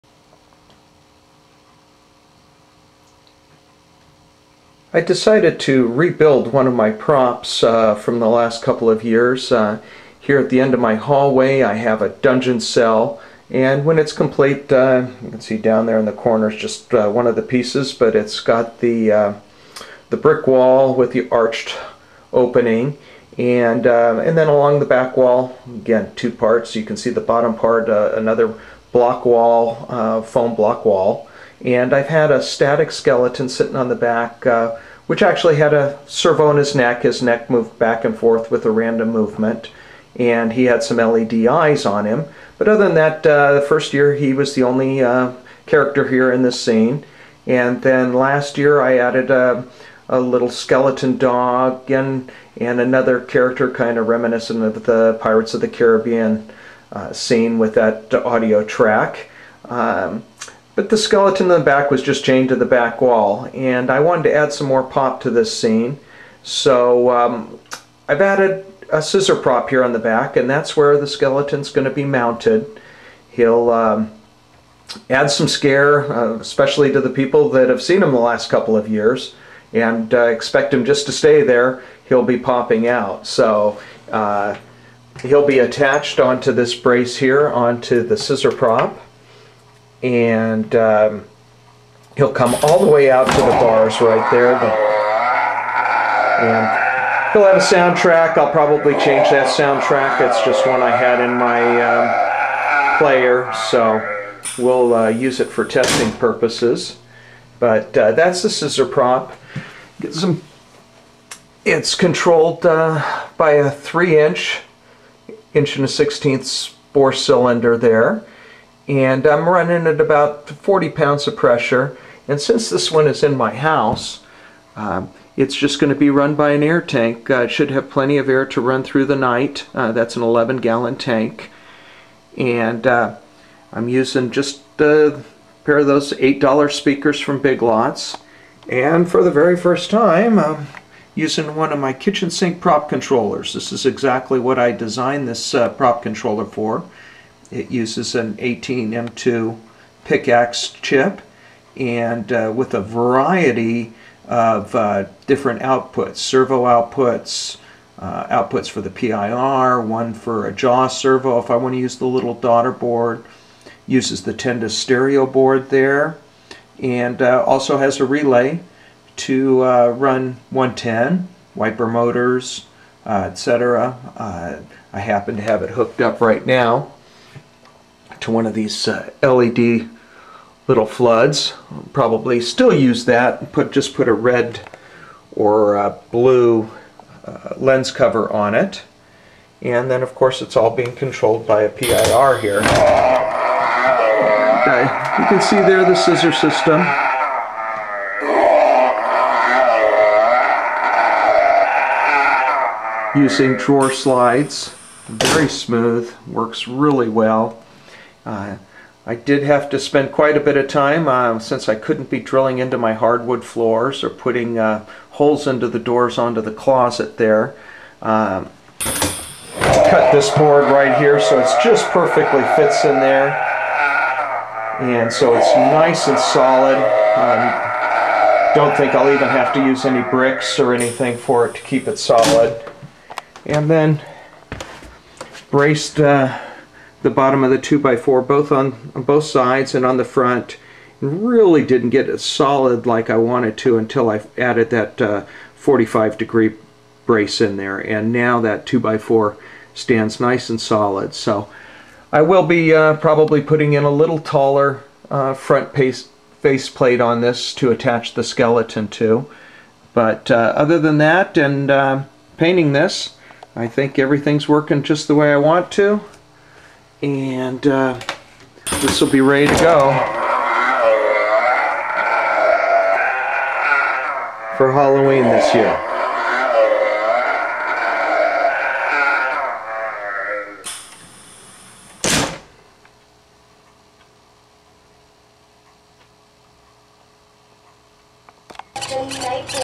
Pneumatic Scissor Halloween Prop
This Pneumatic Scissor Halloween Prop is an upgrade to a prop that I felt needed some more action so I've added a pneumatic scissor to it.  It's controlled by a Picaxe controller I designed with multiple inputs and outputs so that it can be used for just about any prop I design.

If you liked this video and want more info on building pneumatic props, make sure to check out the playlist on all the pneumatic prop videos